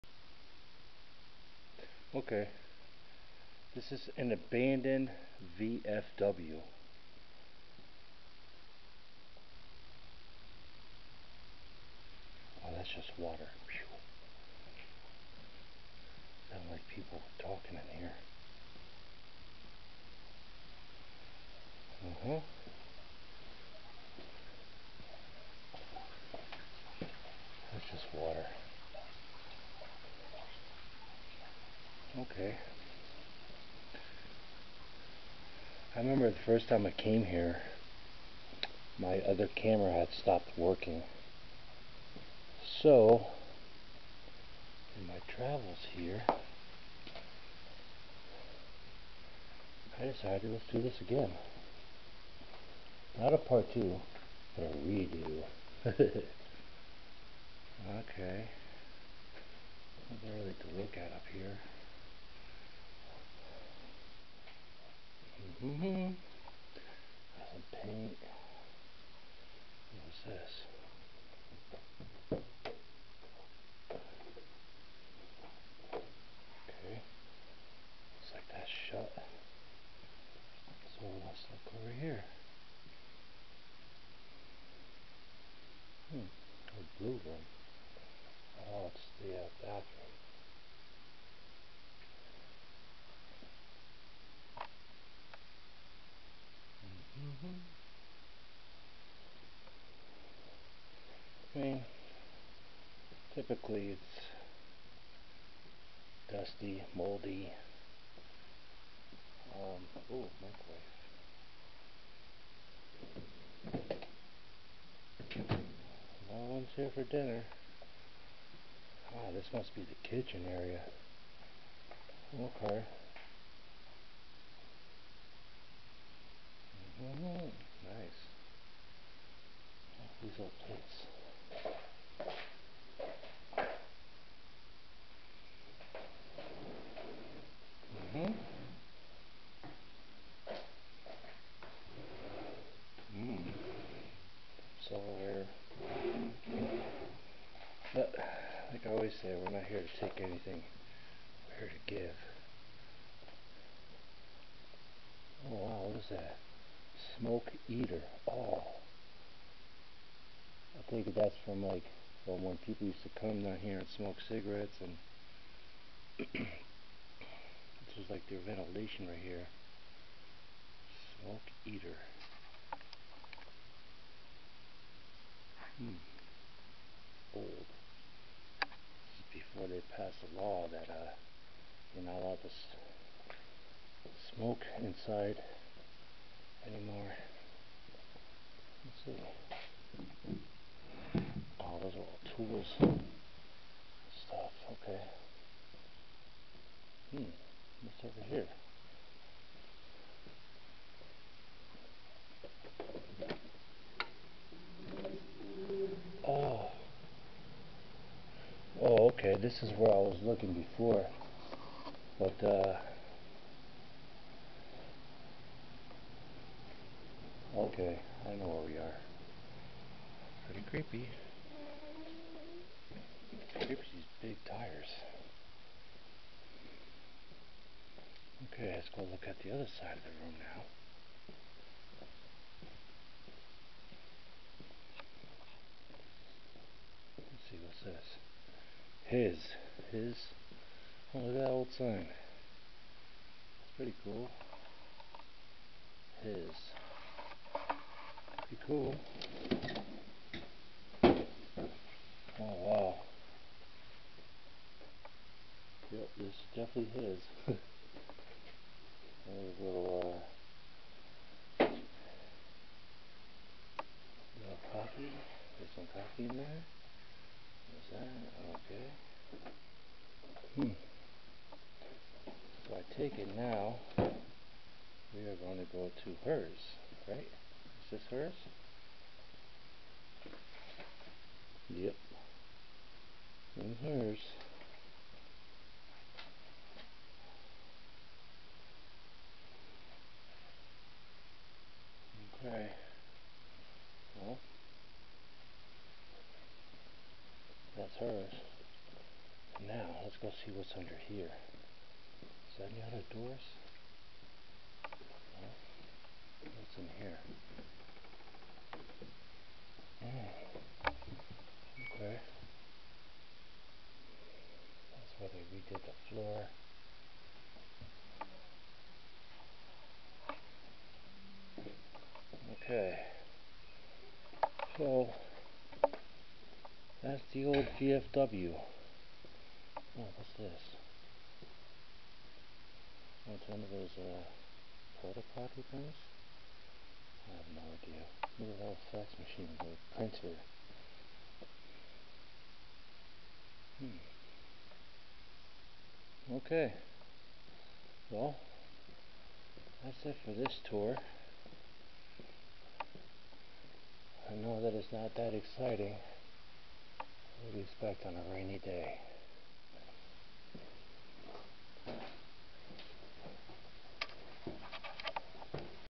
1 abandoned V.F.W - post 603 basement / the SMOKEETER
basement of an abandoned V.F.W.- post 603

open for suggestions & comments